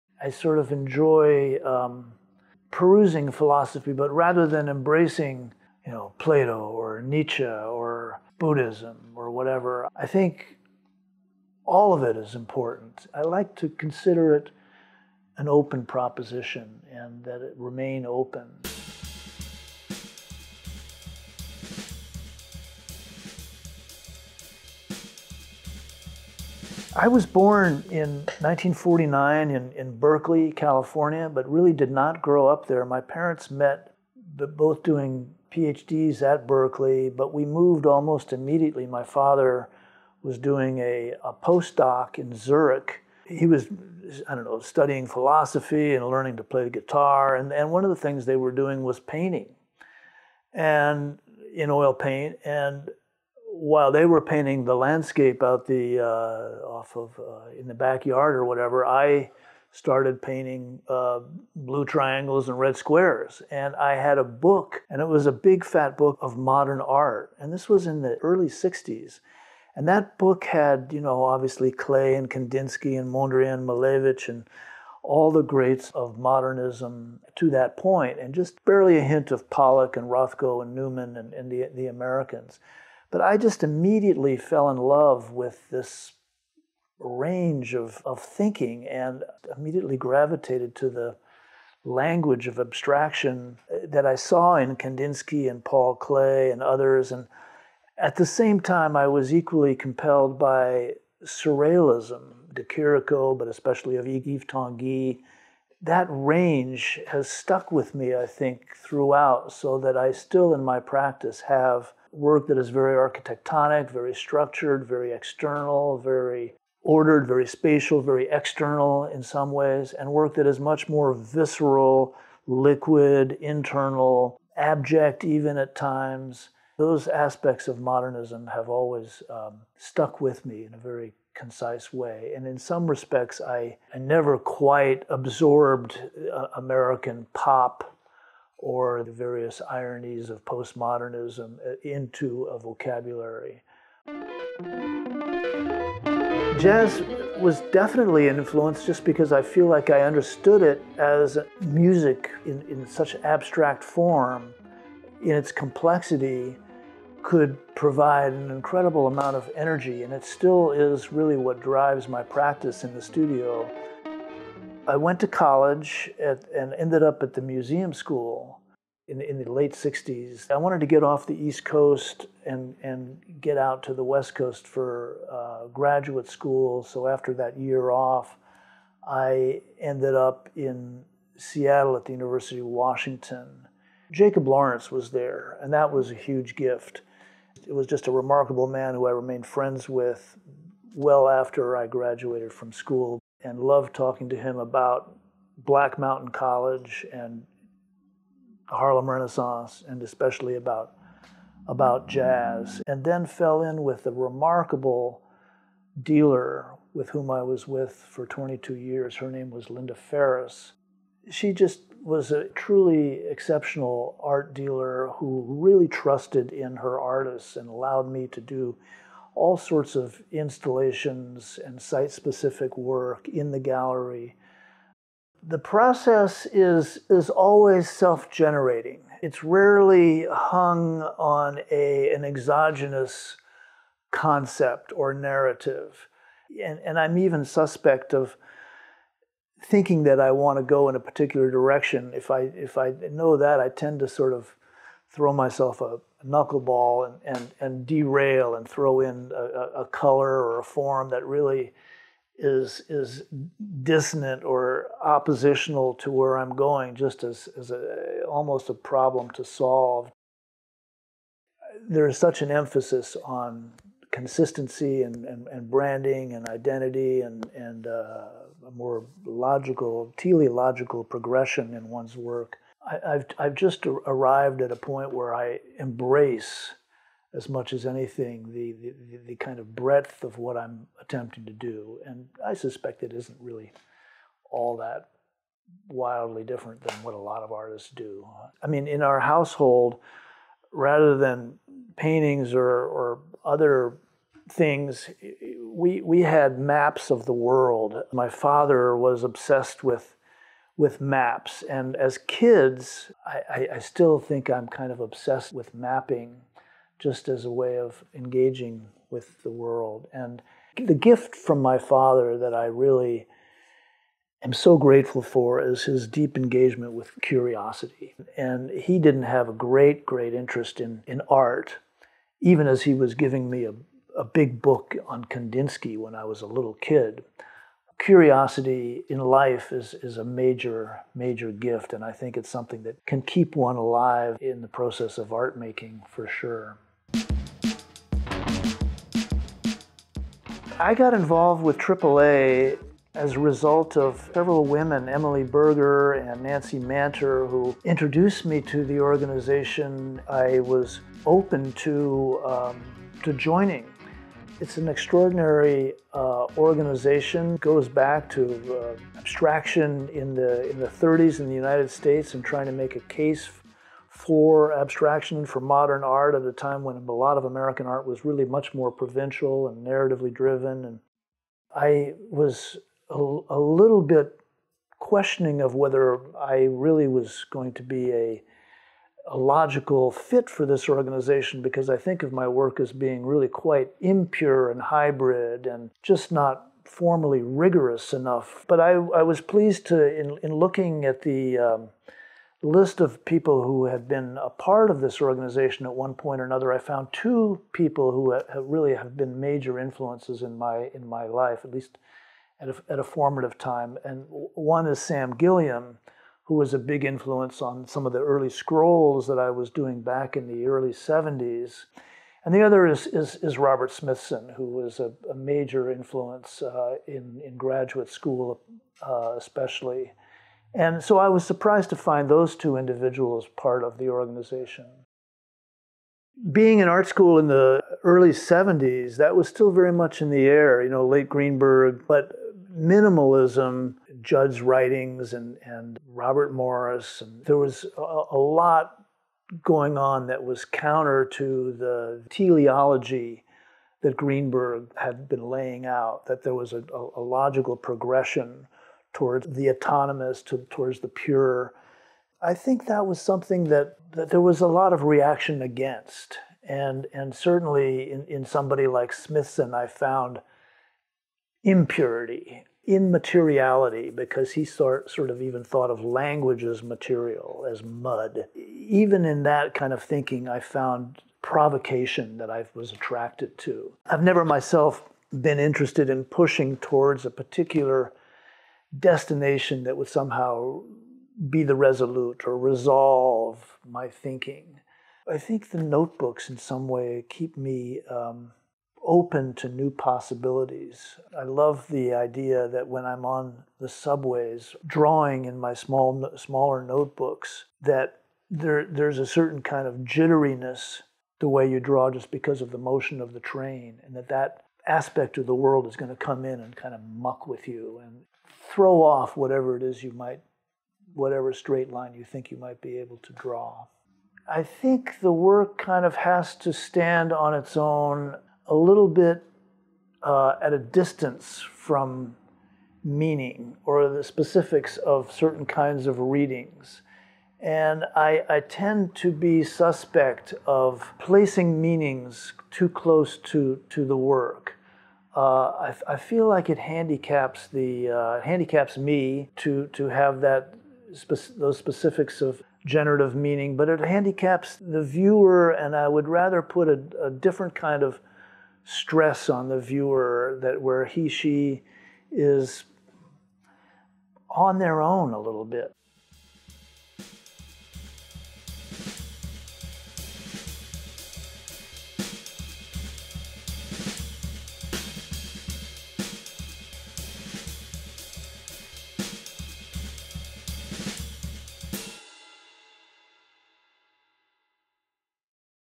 In the Studio with Jeffrey Bishop
AAA member and artist Jeffrey Bishop talks about his range of work and exploration from his studio in Brooklyn NY.